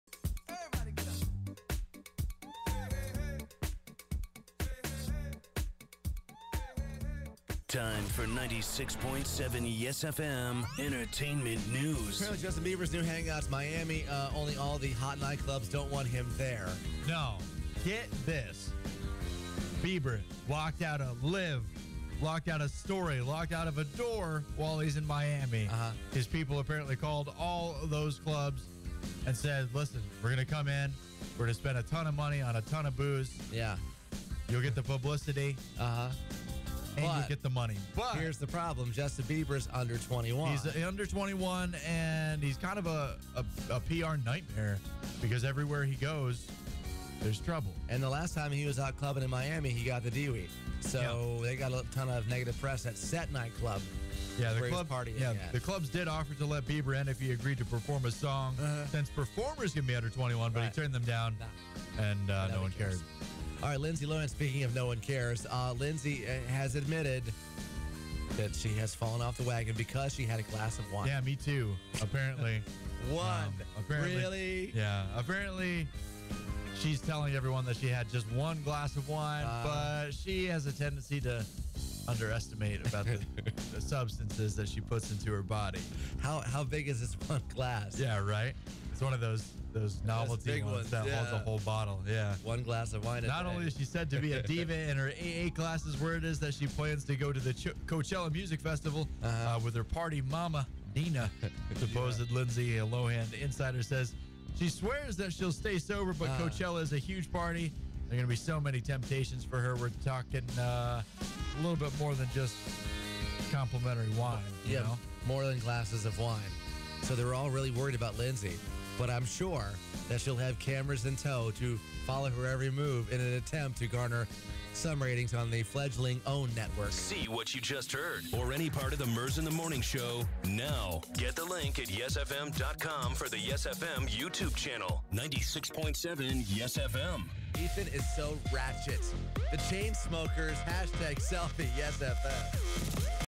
YES-FM Entertainment News Tuesday
Justin Bieber banned from Miami nightclubs and Linsday had a (gasp!) glass of wine!  Uh oh!